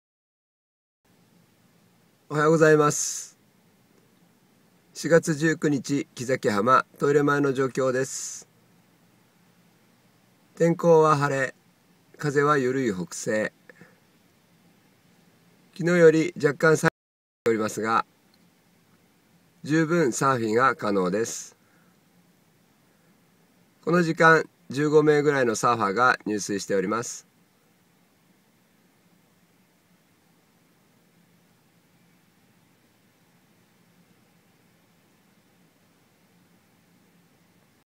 木崎浜 4/19 6:30
天候は晴れ、風はゆるい北西
サイズは腰〜胸くらいで十分アクションも可能です！！
大潮 干潮は11:18,23:32、満潮は17:41